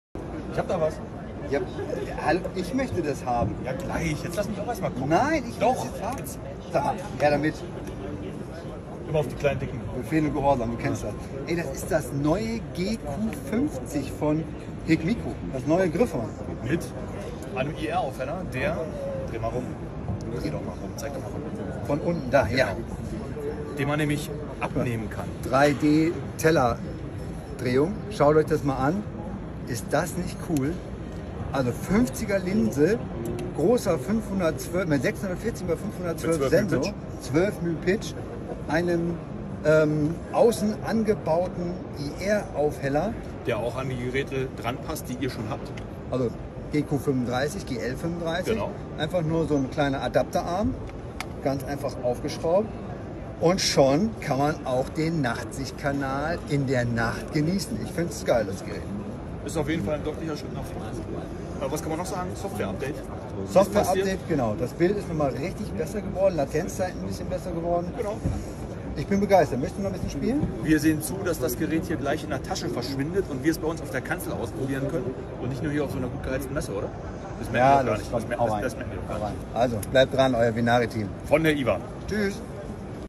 HIKmicro Gryphon GQ50L | Wärmebildkamera auf der IWA OutdoorClassics vorgestellt
Eckdaten zum Gryphon GQ50L
➤ Vorgestellt auf der IWA Outdoor Messe 2022
➤ 50mm Linse für den Wärmebildsensor – besonders groß!
➤ Separates Nachtsicht-Modul – somit sowohl Wärmebild als auch Nachtsicht (Infrarot)-fähig
➤ Zusammen ergibt sich ein fusioniertes Bild (“Fusions-Technologie“) aus Wärmebild & Nachtsicht für besonders hohe Bilddetails und bessere Einschätzung der Situation
➤ Besonders lange Garantie bei VENARI | 3 Jahre Hersteller-Garantie
➤ Integrierter Laser-Entfernungsmesser zur exakten Bestimmung der Distanzen zum Zielobjekt
➤ Systemleistung: 35mm Objektivdurchmesser | OLED Display: 1.024 x 768 Bildpunkte
➤ Sensor: 640 x 512 Bildpunkte und 12 Micrometer Pixelintervall
➤ Sehr gute Ausstattung umfassend: Video- & Fotoaufnahme | WLAN & App Unterstützung | Bild-in-Bild Funktion (PIP) | Höhenmesser | Digitaler Kompass | Schwerkraftsensor